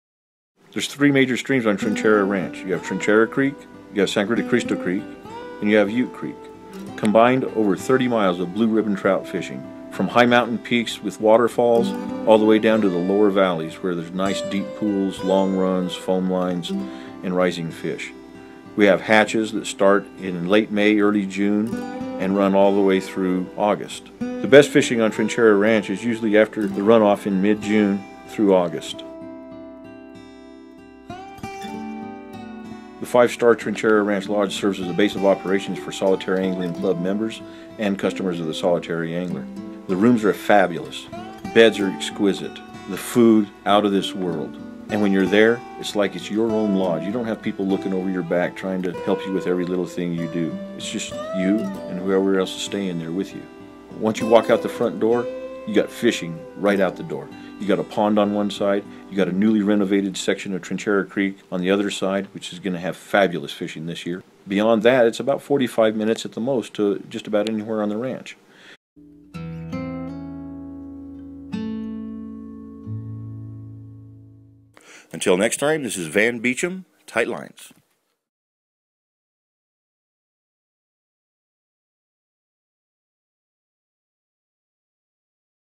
The Solitary Angler at Trinchera Ranch
Solitary Angler's Quickstart School with Van Beacham & Bob Wid­gren
Hello anglers, this is Van Beacham with some news I'm very excited about! Bob Wid­gren of Los Pinos Rods and I will be con­duct­ing two "Quick Start" Fly Fish­ing Schools this year at the fab­u­lous Trinchera Ranch. Between the two of us you will learn more about fly fish­ing in two days than you can shake a "rod" at!